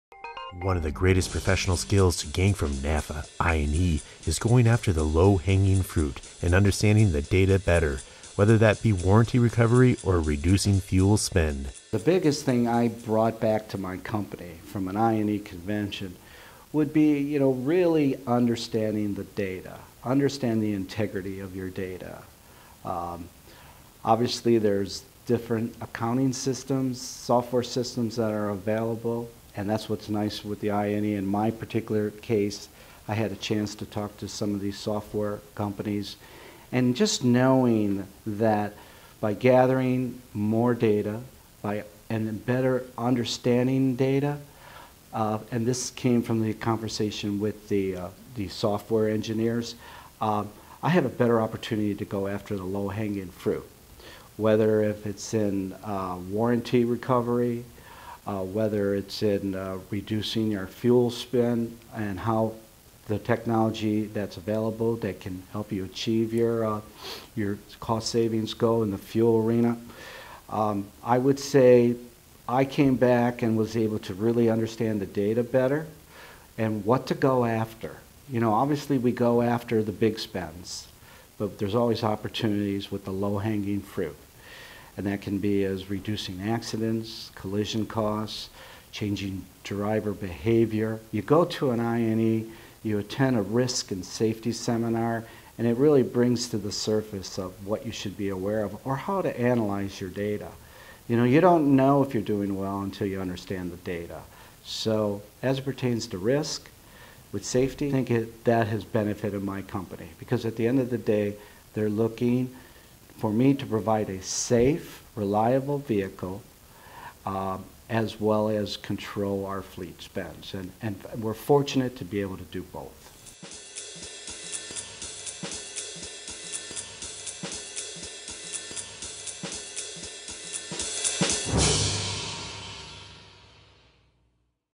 Understanding the Integrity of Your Data | John Dmochowsky | AIN
One of the greatest professional skills to gain from NAFA I&E is going after the low-hanging fruit and understanding data better, whether that be warranty recovery or reducing fuel spend.

John Dmochowsky
Sr. Sales Fleet Manager
Mandelez International

Ain_01_25_2013_Nafa_BofGvnrs_John_Dmochowsky _VC2.mov